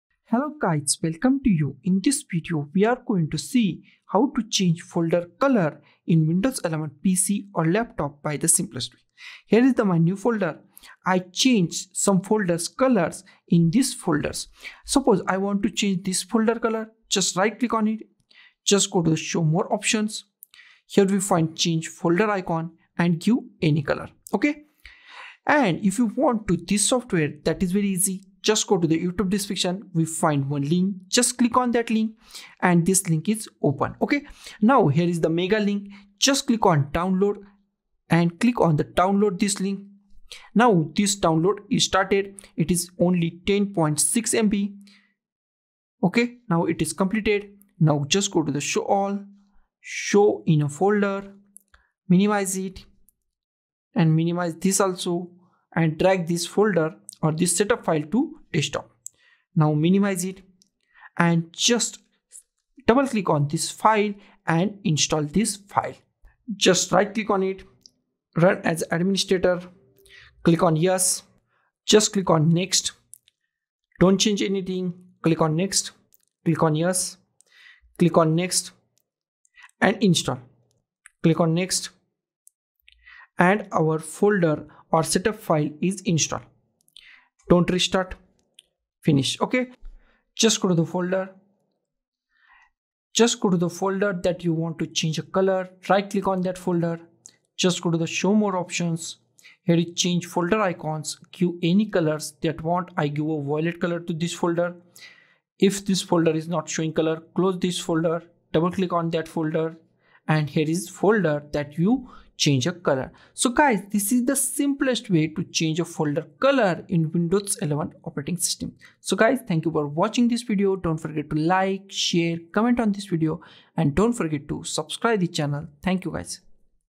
How to Change Folder Color in Windows 11
In this Video we are going to see How to Change Folder Color in Windows 11 PC or Laptop by the simplest Way...